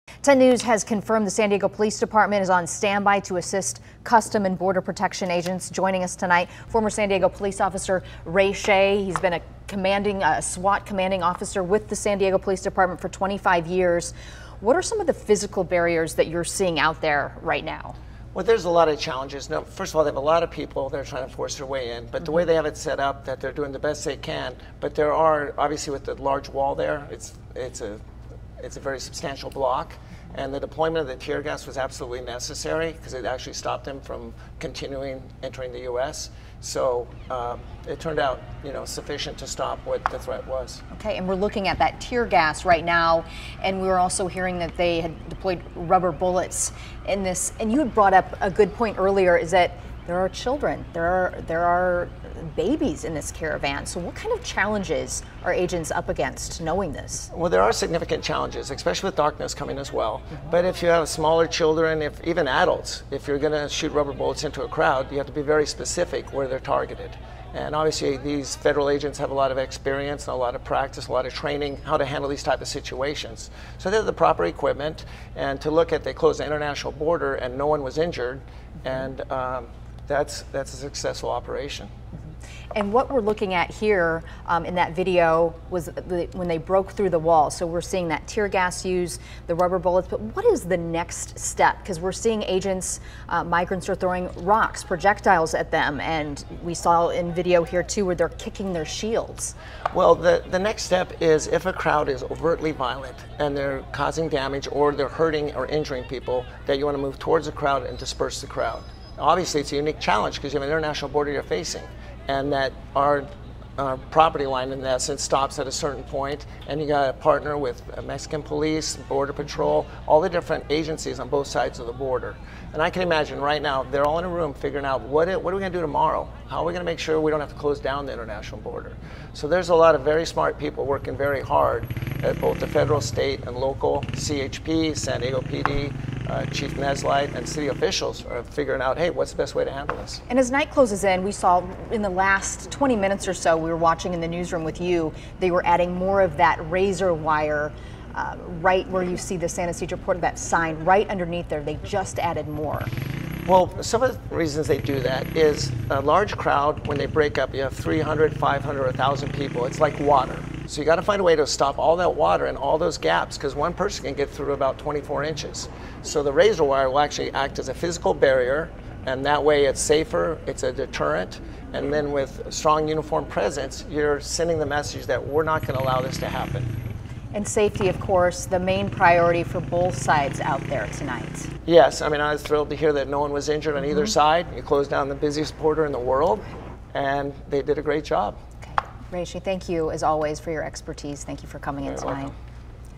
SDPD on standby to join border effort
Agents fired tear gas on migrants.